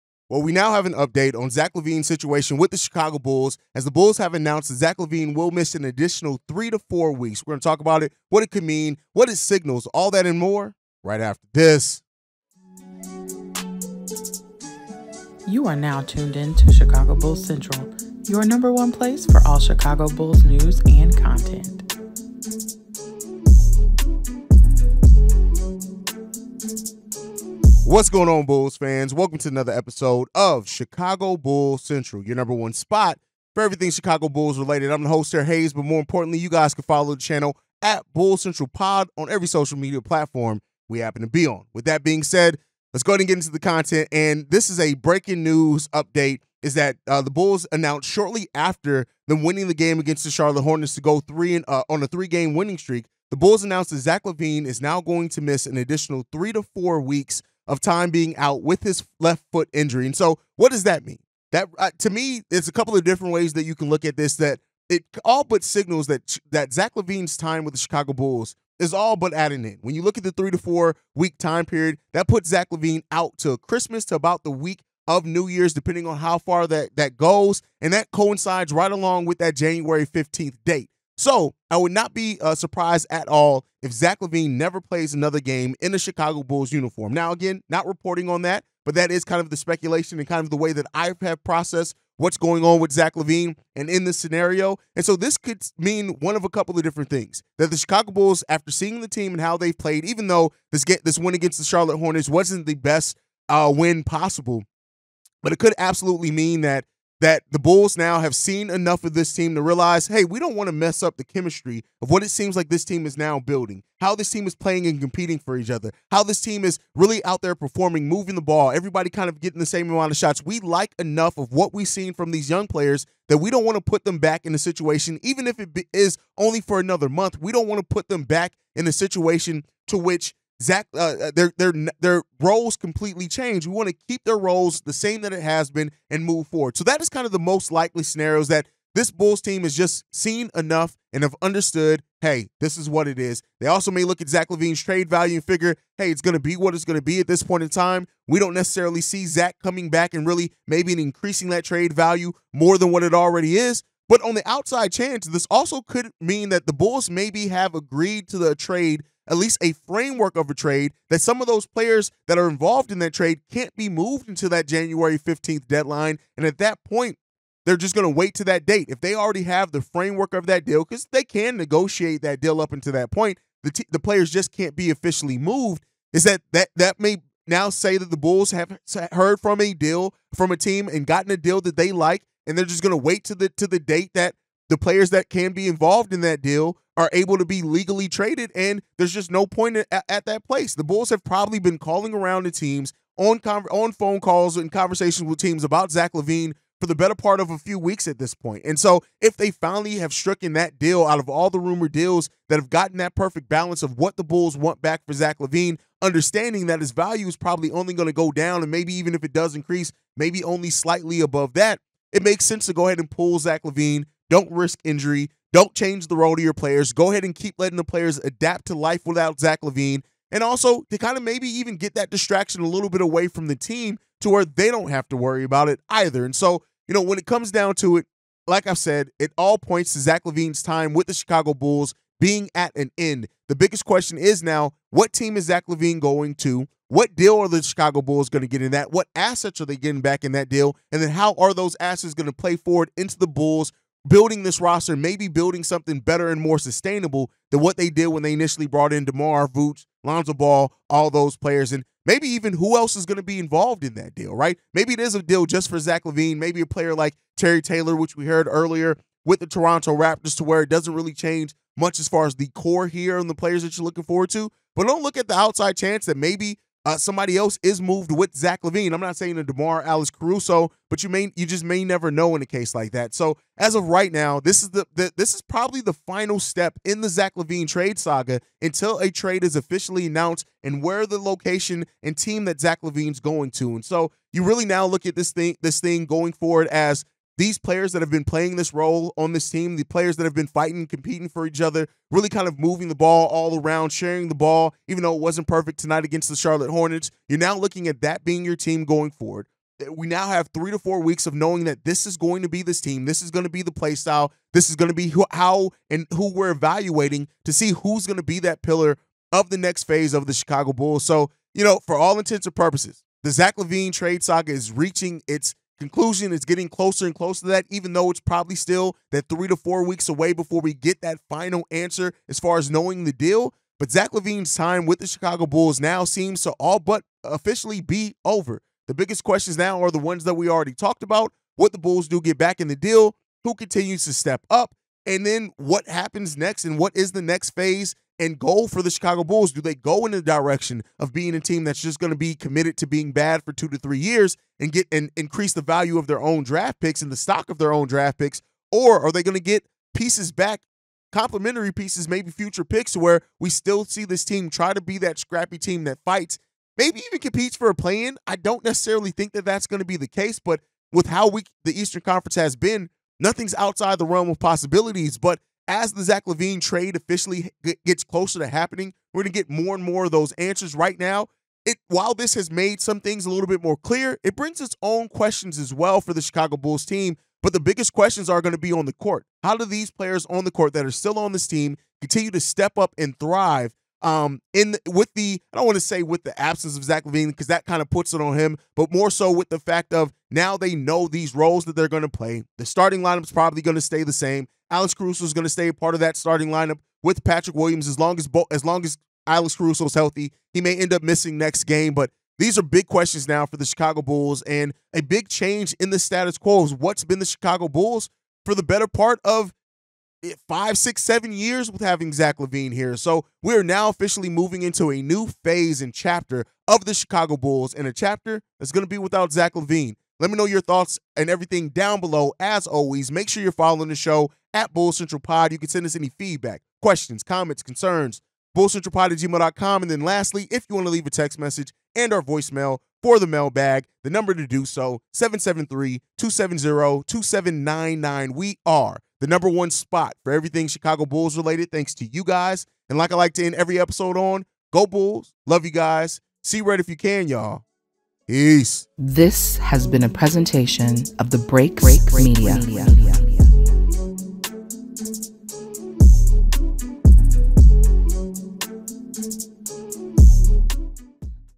Breaking News: Chicago Bulls Announce Zach LaVine Will Be Out 3-4 Is Time With Team Officially Over?
Haize reacts to the news that Zach LaVine will be out another 3-4 weeks adding to speculation that he has played his last game in a Bulls jersey

THE VOICE OF THE FANS!